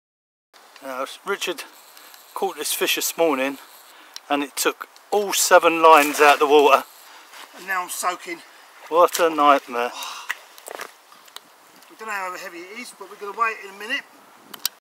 a 50lb sturgeon in france
this was at a lake in france. i am going back to france in june. i hope to bring back a new video